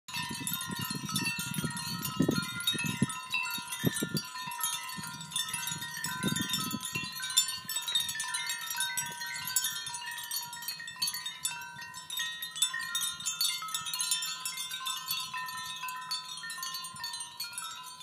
Just Tinkling In The Wind - Bottles glass chimes 2
Video of bottles with glass chimes.